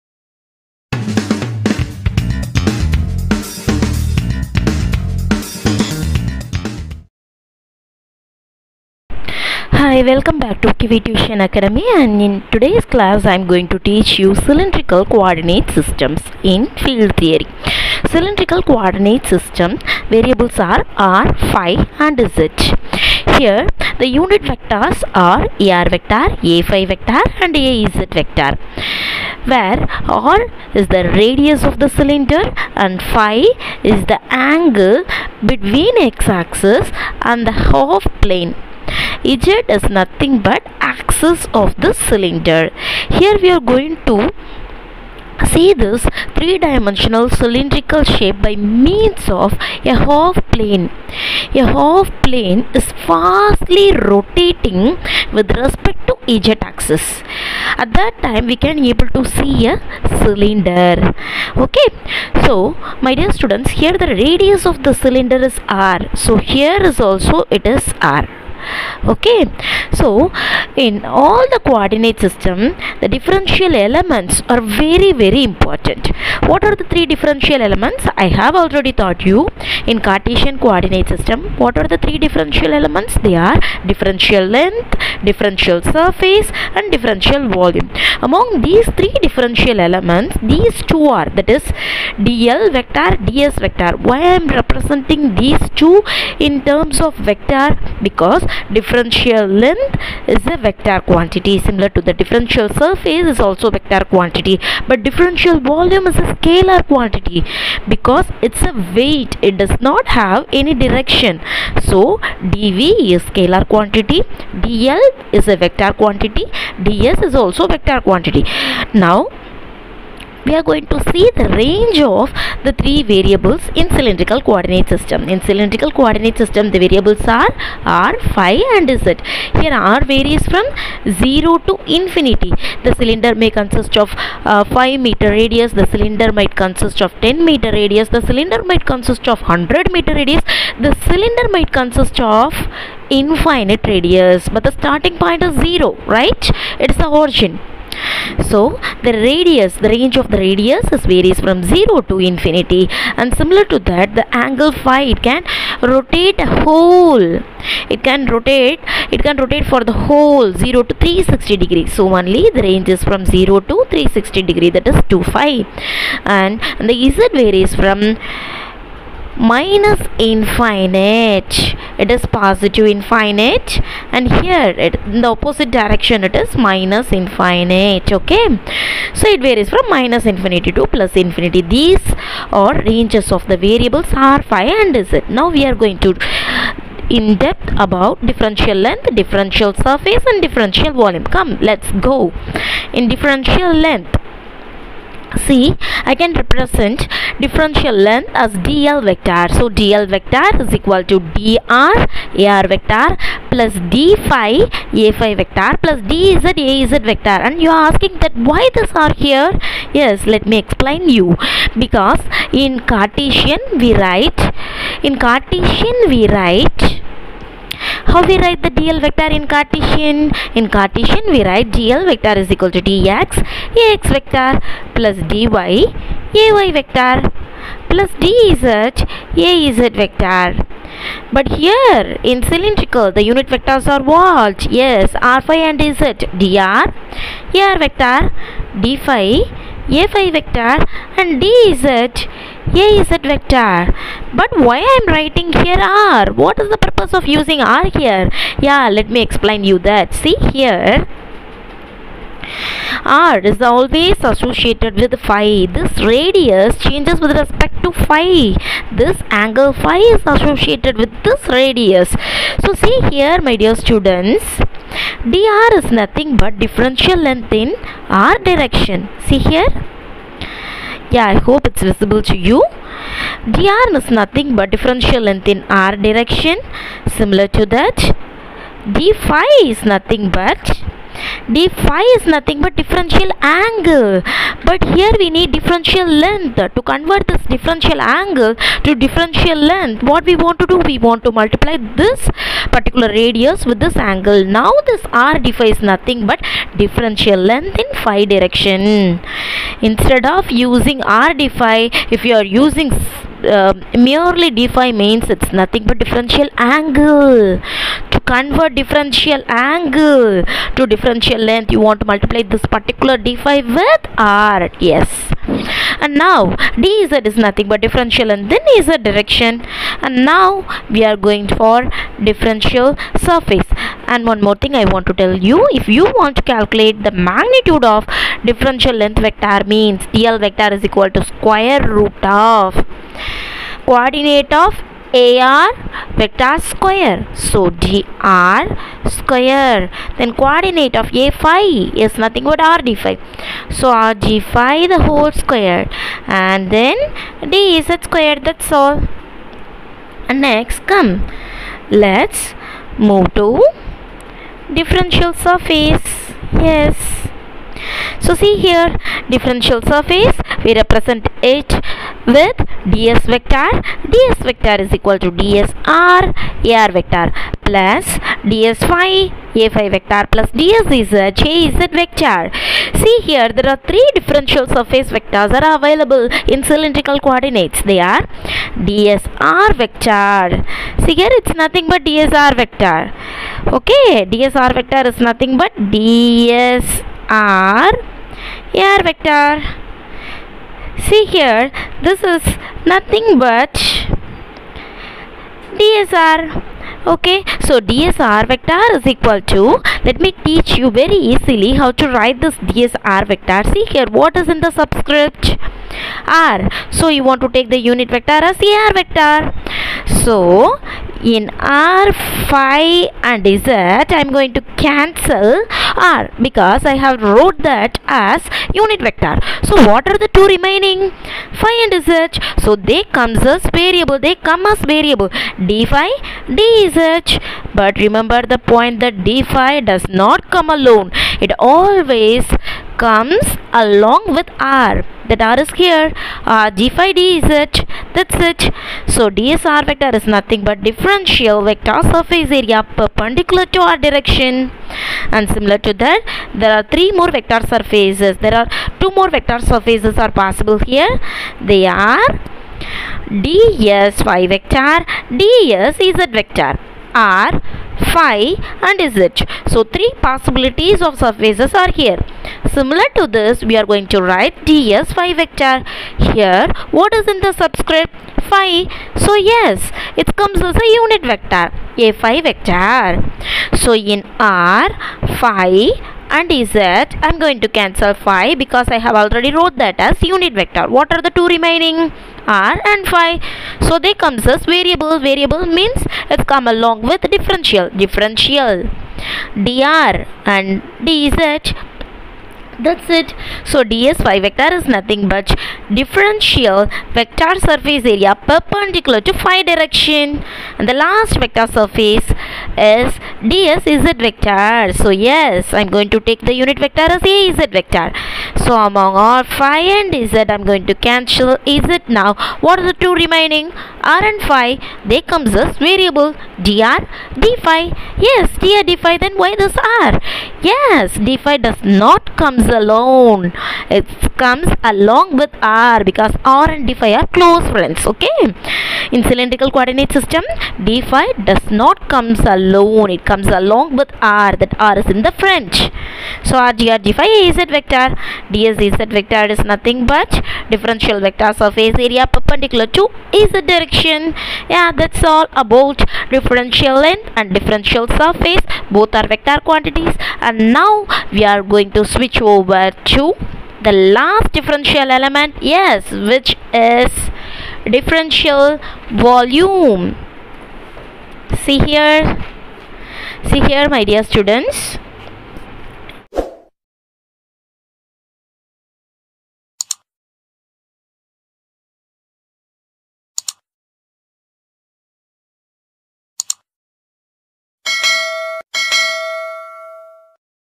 Cylindrical Coordinates in Electromagnetic Fields|தமிழ்|Kiwi Tuition Academy
In this video, we have clearly stated that the basic concepts of Cylindrical Coordinate Systems.